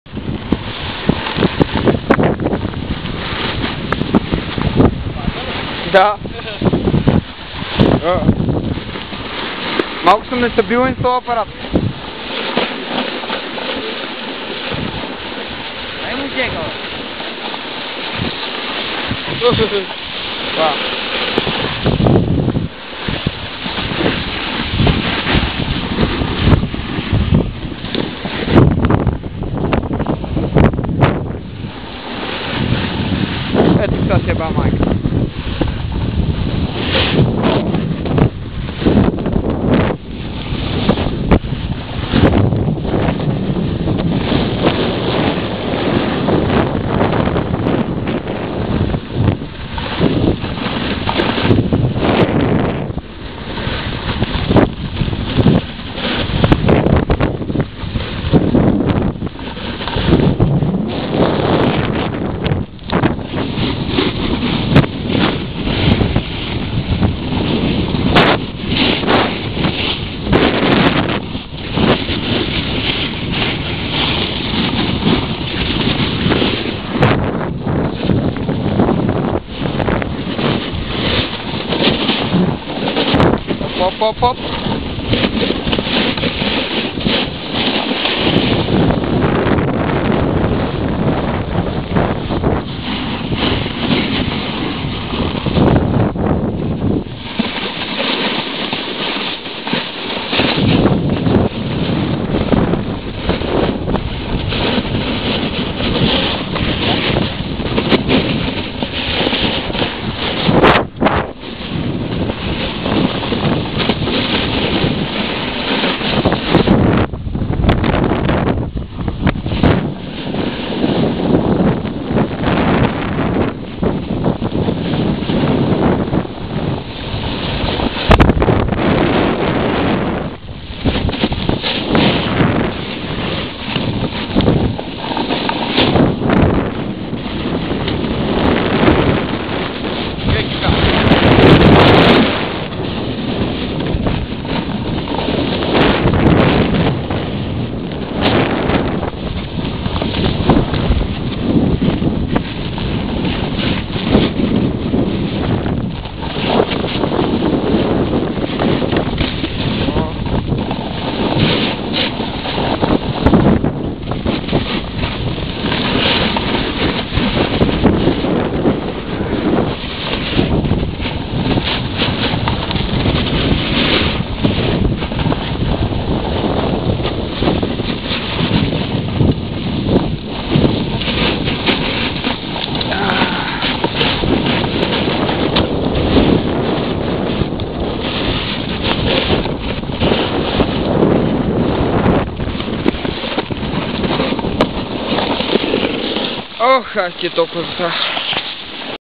Chepelare ski run - Bulgarian Mountains
Riding on the Chepelare mountain Mechi Chal with my friends !!!